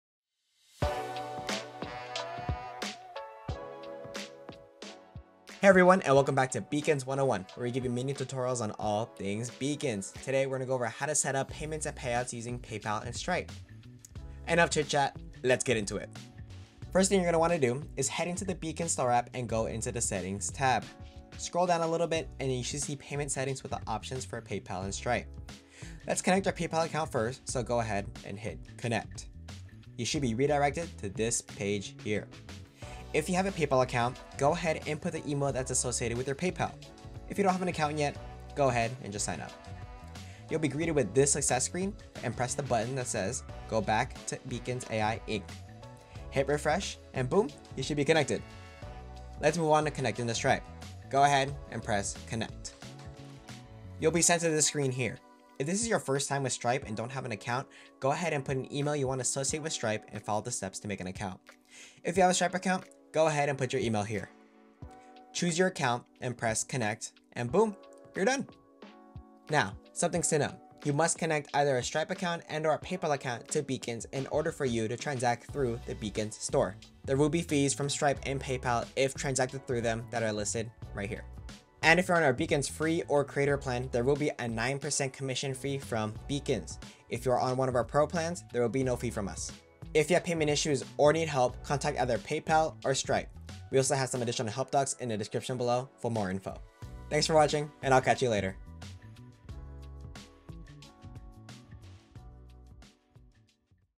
How to Setup Payments & Payouts on Beacons! (Tutorial)
Welcome back to Beacons 101 where we give you quick tutorials with Tips & Tricks on all things Beacons!

Todays tutorial is how to how to connect your paypal and stripe account to Beacons to accept payments and payout to get paid 🤑

🕐  Timestamps  🕐

0:00 - Introduction 🎬
0:17 - Setting up Paypal 💰
0:49 - Setting up Stripe 💸
1:09 - Things to Note 📝
1:30 - Ending 🏁